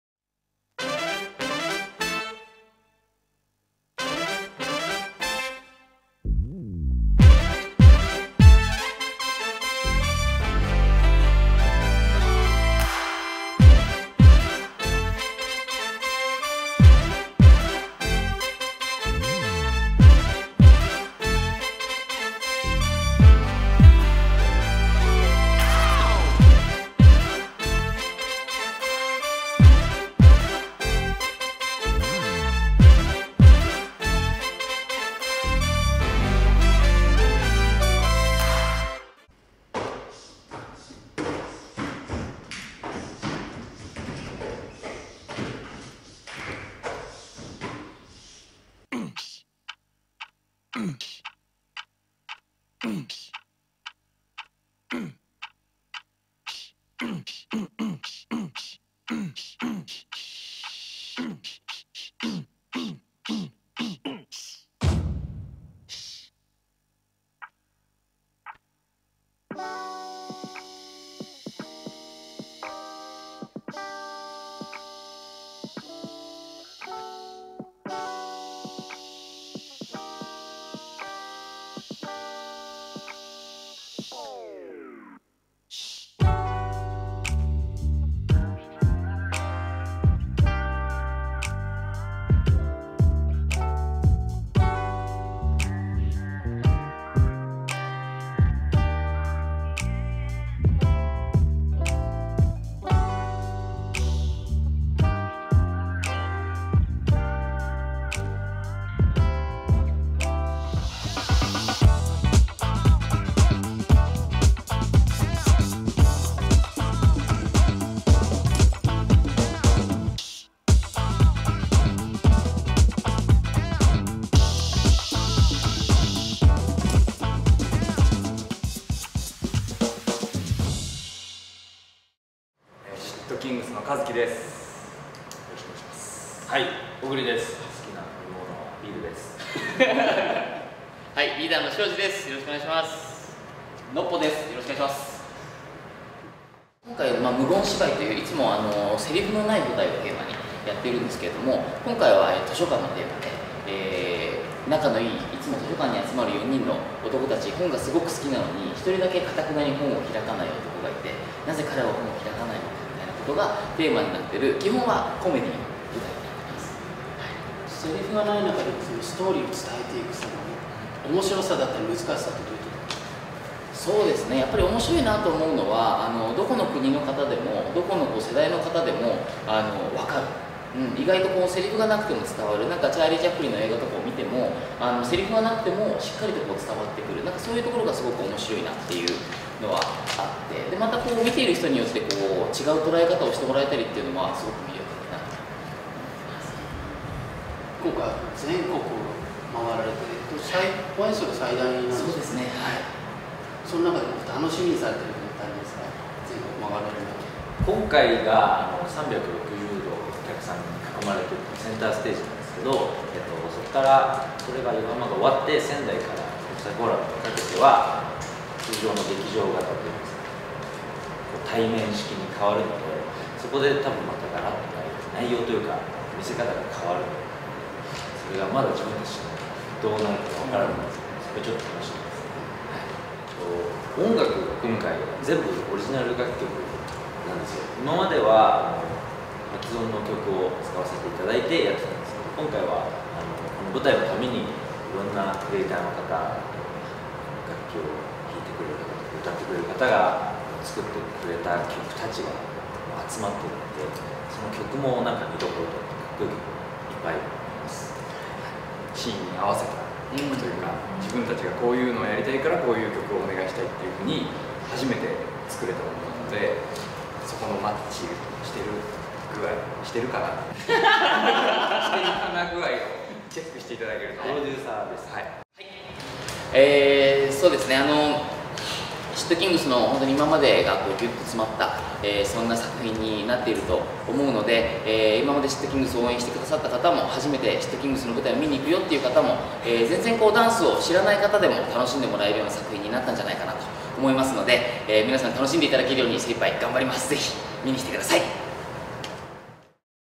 【動画レポ：【s**t kingz】全国ツアー『The_Library』ゲネプロ＆取材会ダイジェスト】
4人組ダンスパフォーマンスグループ【s**t kingz】（シットキングス）、2年ぶりとなる待望の新作舞台公演『The_Library』は2 か月以上をかけ過去最大規模の、全国 7 都市29公演に加え、初日プレビュー公演にて追加公演（東京国際フォーラム ホール C）のスケジュールが発表され、全30公演となる全国ツアーを、9月12日に横浜公演（横浜 赤レンガ倉庫1号館）からスタートさせました。

10 周年を迎えた【s**t kingz】、ダンスで魅せる"無言芝居、『The Library』のゲネプロと初日に行われた囲み取材よりダイジェスト映像をお届けします！

【あらすじ】
不思議な図書館に集まる４人の男たち。
真面目に読書をしていたかと思えば、遊んでふざけて、怒られる事もしばしば。
それでも、４人は図書館に集まり本を読む事が大好きだった。本をかたくなに開こうとしない”１人の男”を除いては・・・。

■公演概要
s**t kingz『The Library』
9月12日（水）〜 9月20日（木）神奈川・横浜 赤レンガ倉庫1号館 3階ホール
10月10日（水）11日（木）：宮城・仙台銀行ホール イズミティ21
10月20日（土）21日（日）：愛知・穂の国とよはし芸術劇場
10月27日（土）28日（日）：広島・広島JMSアステールプラザ中ホール　※昼夜2公演
11月3日（土）4日（日）：福岡・ももちパレス・大ホール　※昼夜2公演
11月9日（金）10日（土）11日（日）：大阪・サンケイホールブリーゼ
11月21日（水）22日（木）23日（金・祝）：東京・東京国際フォーラム ホールC

💛s**t kingz （グッズ通販：A!SMART）